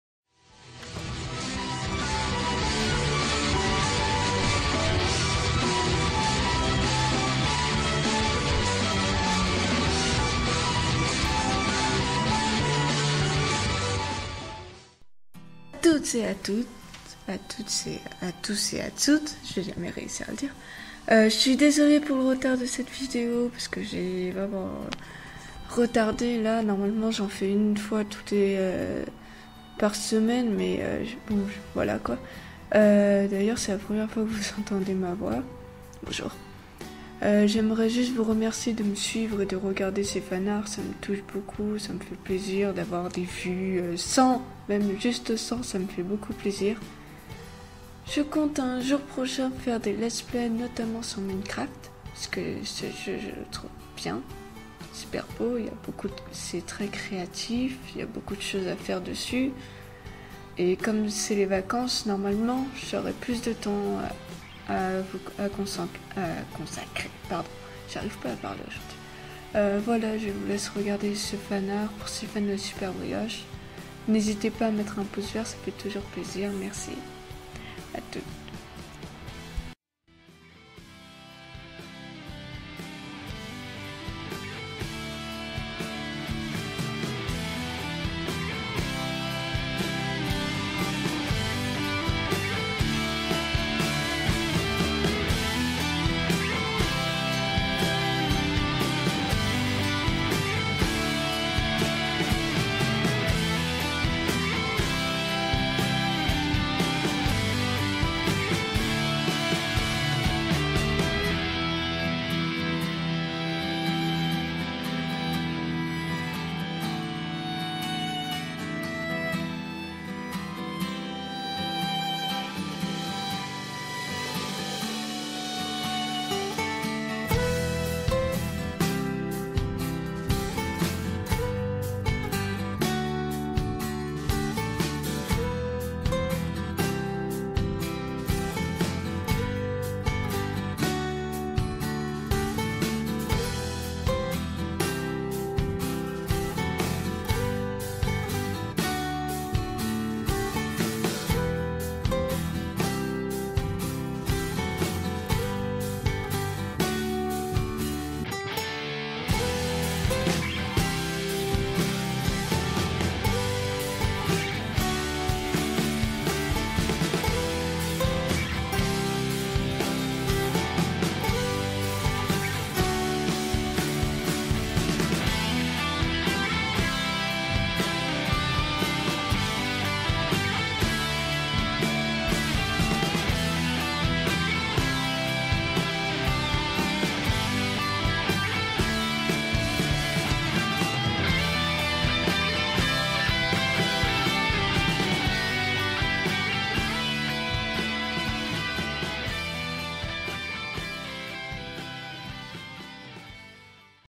Fan art_Siphano & SuperBrioche_L'île au trésor
Voila un fan art un peu différents pour Siphano et Super brioche dans leurs série l'île au trésor. Différents parce que je vous parles un peu dans cette vidéo. Désolé pour certains défauts et le bruit de fond pas super. J'ai enregistré avec mon portable donc voilà ^^".